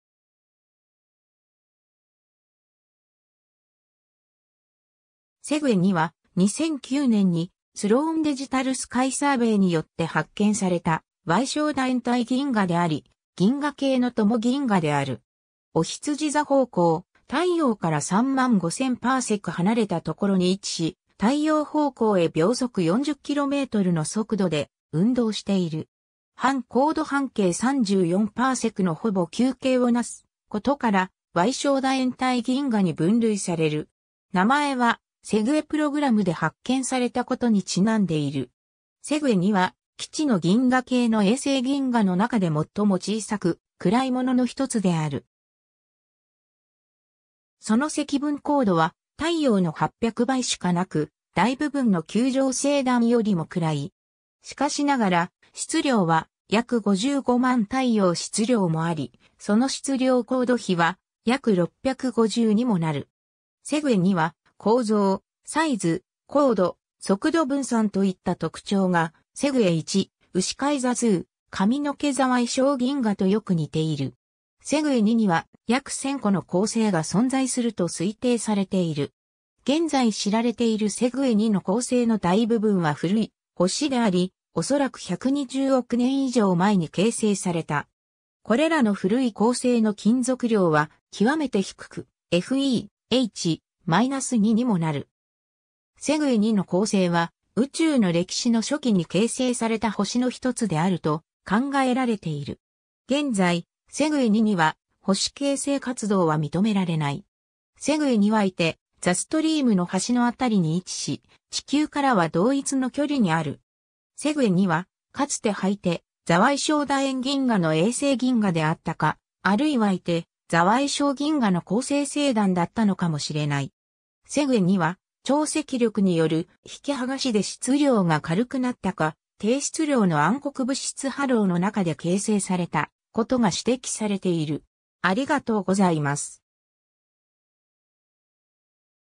SEGUE 2
Segue 2は、2009年にスローン・デジタル・スカイサーベイによって発見された矮小楕円体銀河であり、銀河系の伴銀河である。
おひつじ座方向、太陽から35,000パーセク離れたところに位置し、太陽方向へ40km/sの速度で運動している。
半光度半径（有効半径）34パーセクのほぼ球形をなすことから、矮小楕円体銀河 (dSph) に分類される。
名前は、SEGUE (Sloan Extension for Galactic Understanding and Exploration) プログラムで発見されたことにちなんでいる。
 Segue 2 は、既知の銀河系の衛星銀河の中で最も小さく暗いものの一つである。
その積分光度は太陽の800倍（絶対視等級で約-2.5等に相当）しかなく、大部分の球状星団よりも暗い。
しかしながら、質量は約550,000太陽質量もあり、その質量光度比は約650にもなる。
 Segue 2は構造、サイズ、光度、速度分散といった特徴がSegue 1、うしかい座II、かみのけ座矮小銀河とよく似ている。
 Segue 2には約1,000個の恒星が存在すると推定されている。
現在知られているSegue 2の恒星の大部分は古い星であり、おそらく120億年以上前に形成された。
これらの古い恒星の金属量は極めて低く、[Fe/H] 「 -2 にもなる。
Segue 2の恒星は宇宙の歴史の初期に形成された星の一つであると考えられている。
現在、Segue 2には星形成活動は認められない。
 Segue 2はいて座ストリームの端の辺りに位置し、地球からは同一の距離にある。
Segue 2は、かつてはいて座矮小楕円銀河の衛星銀河であったか、あるいはいて座矮小銀河の構成星団だったのかもしれない。
 Segue 2は、潮汐力による引き剥がしで質量が軽くなったか、低質量の暗黒物質ハローの中で形成されたことが指摘されている。